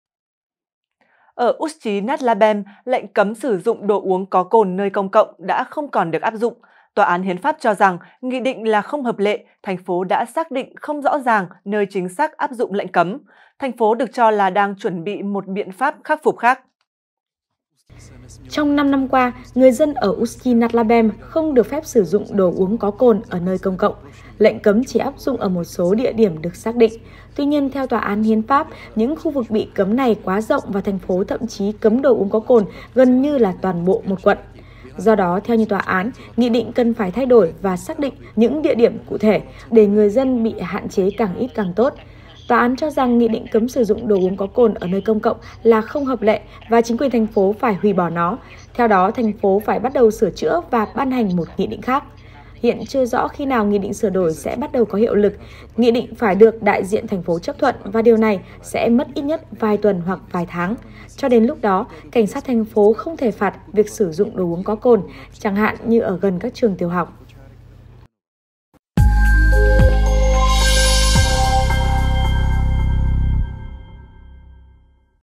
Ústí nad Labem hủy bỏ lệnh cấm uống rượu bia nơi công cộng | Tamdamedia
Ở Ústí nad Labem, lệnh cấm sử dụng đồ uống có cồn nơi công cộng đã không còn được áp dụng. Tòa án Hiến pháp cho rằng, nghị định là không hợp lệ, thành phố đã xác định không rõ ràng nơi chính xác áp dụng lệnh cấm. Thành phố được cho là đang chuẩn bị một biện pháp khắc phục khác.